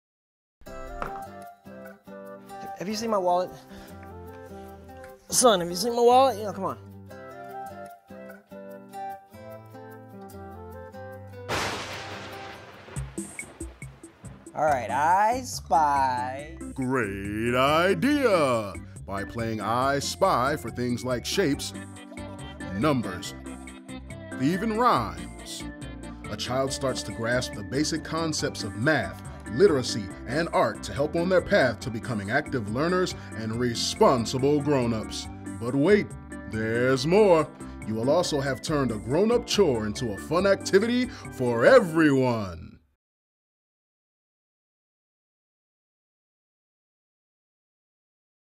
I Spy
Play "I Spy" with shapes, rhymes, and colors to build early academic skills in a fun way.